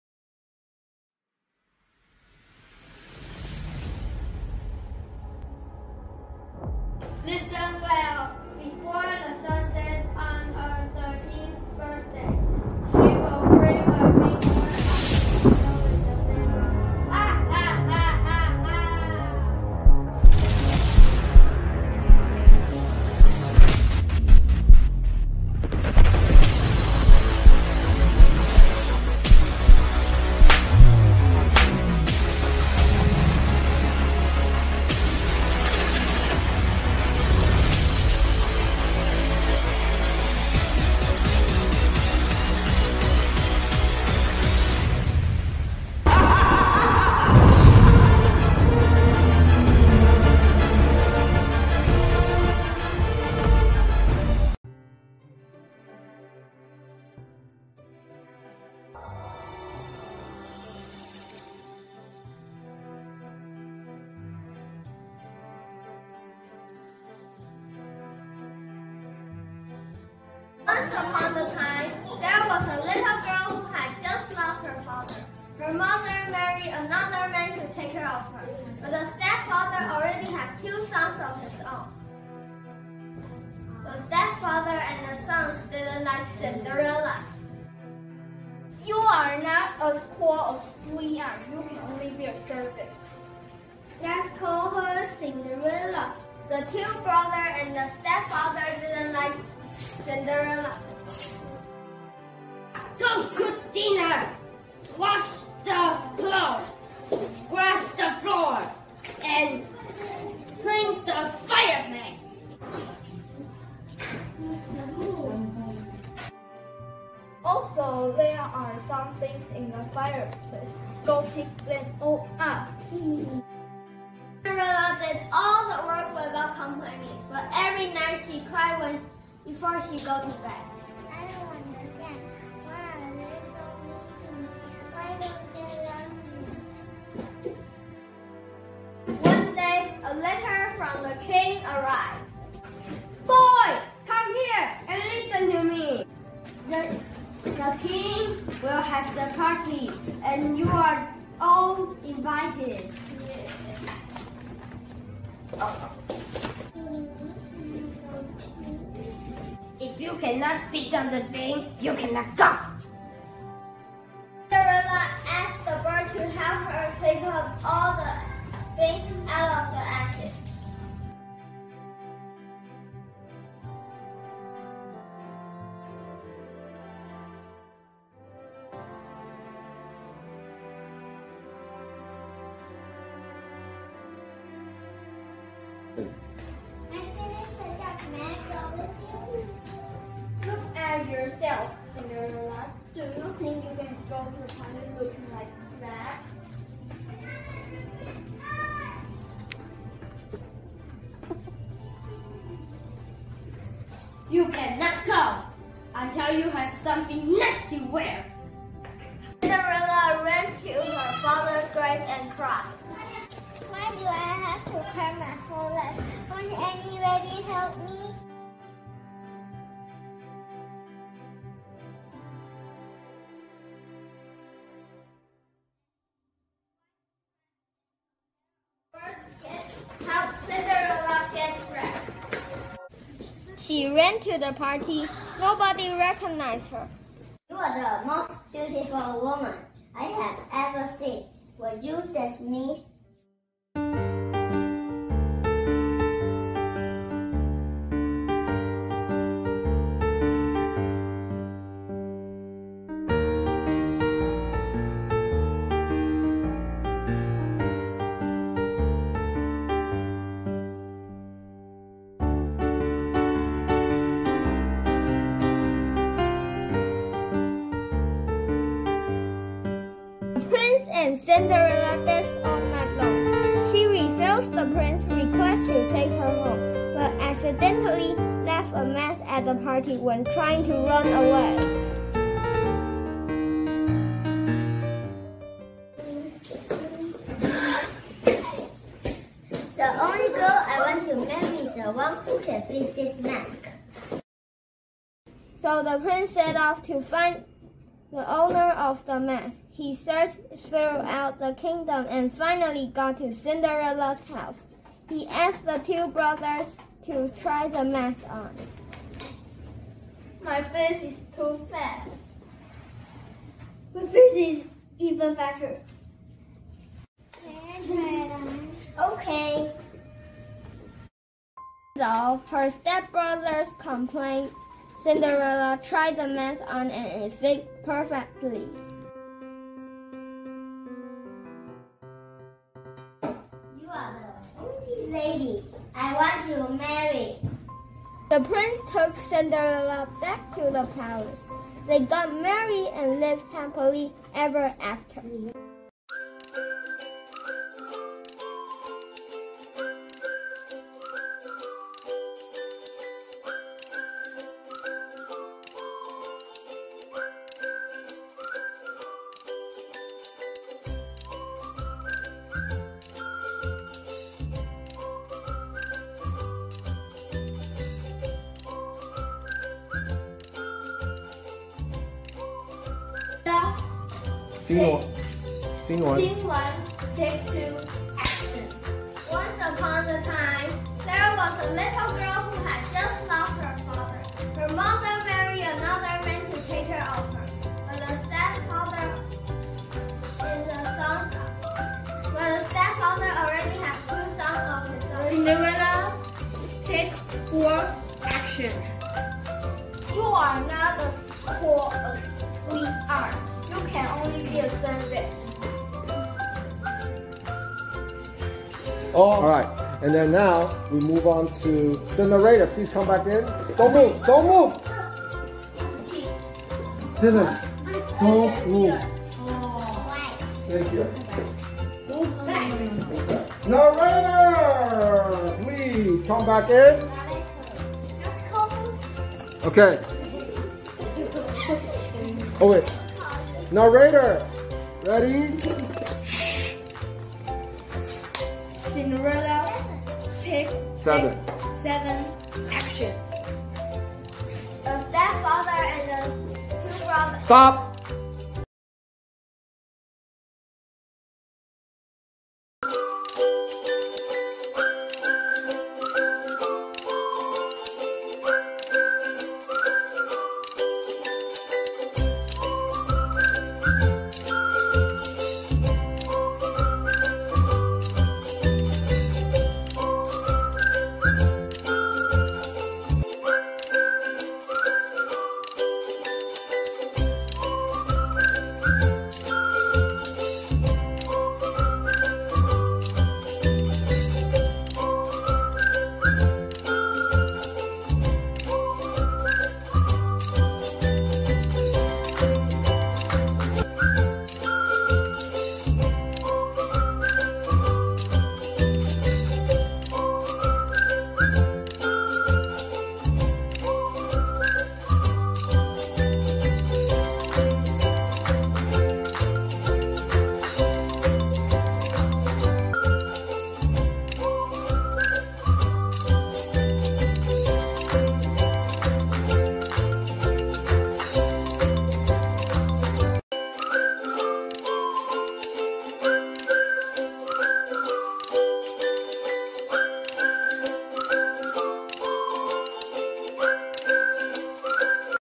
2022 ShiPai Mingde Wonderful Summer Movie Project
The students were having fun filming the movies, making the props and writing the lines for their movies~ This is one of the best summer programs that we have ever had.